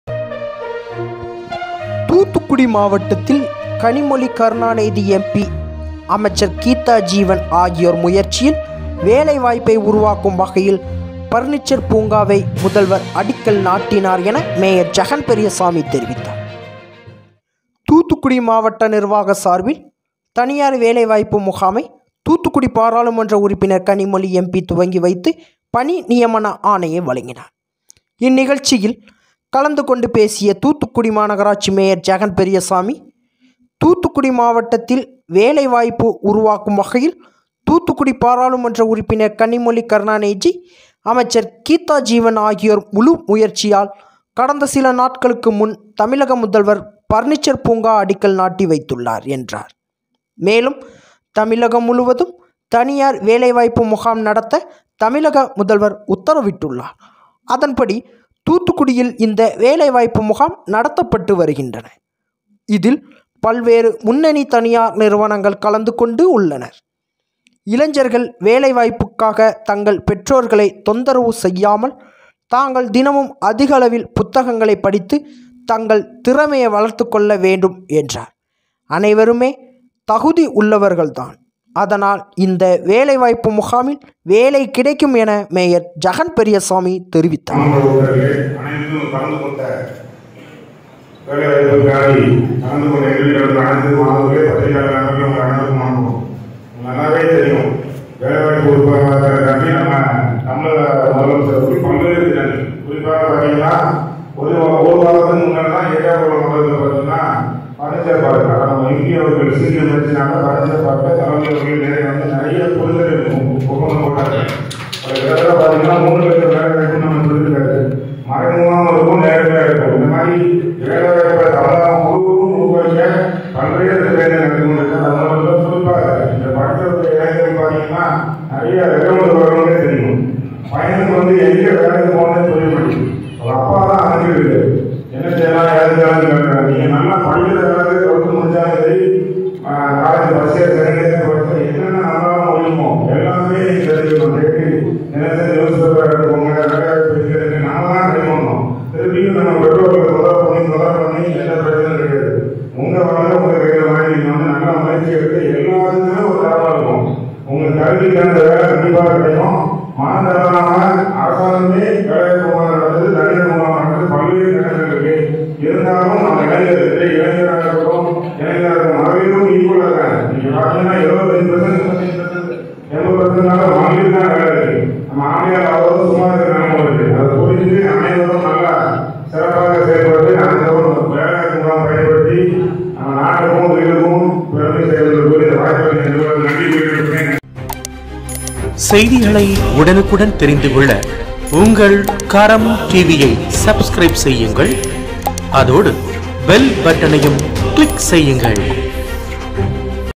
வேலைவாய்ப்பு உருவாக்கும் முதல்வர்: மேயர் ஜெகன் பெரியசாமி பெருமிதம்
தூத்துக்குடி மாவட்ட நிர்வாகம் சார்பில் தனியார் வேலைவாய்ப்பு முகாமை தூத்துக்குடி பாராளுமன்ற உறுப்பினர் கனிமொழி எம்பி துவங்கி வைத்து பணி நியமன ஆணையை வழங்கினார்.

 இந்த நிகழ்ச்சியில் கலந்துகொண்டு பேசிய தூத்துக்குடி மாநகராட்சி மேயர் ஜெகன் பெரியசாமி தூத்துக்குடி மாவட்டத்தில் வேலைவாய்ப்பு உருவாக்கும் வகையில் தூத்துக்குடி பாராளுமன்ற உறுப்பினர் கனிமொழி கருணாநிதி அமைச்சர் கீதாஜீவன் ஆகியோர் முழு முயற்சியால் கடந்த சில நாட்களுக்கு முன் தமிழக முதல்வர் பர்னிச்சர் பூங்கா அடிக்கல் நாட்டி வைத்துள்ளார் என்றார்

மேலும் தமிழகம் முழுவதும் தனியார் வேலைவாய்ப்பு முகாம் நடத்த தமிழக முதல்வர் உத்தரவிட்டுள்ளார் அதன்படி தூத்துக்குடியில் இந்த வேலைவாய்ப்பு முகாம் நடத்தப்பட்டு வருகிறது.

 இதில் பல்வேறு முன்னணி தனியார் நிறுவனங்கள் கலந்து கொண்டு உள்ளனர் இளைஞர்கள் வேலைவாய்ப்புக்காக தங்கள் பெற்றோர்களை தொந்தரவு செய்யாமல் தாங்கள் தினமும்  அதிகளவில்  புத்தகங்களை  படித்து  தங்களை திறமையை வளர்த்துக் கொள்ள வேண்டும்  என்றார் அனைவருமே தகுதி உள்ளவர்கள் தான் அதனால்  இந்த வேலைவாய்ப்பு முகாமில் வேலை கிடைக்கும்  என  மேயர் ஜெகன் பெரியசாமி  தெரிவித்தார்.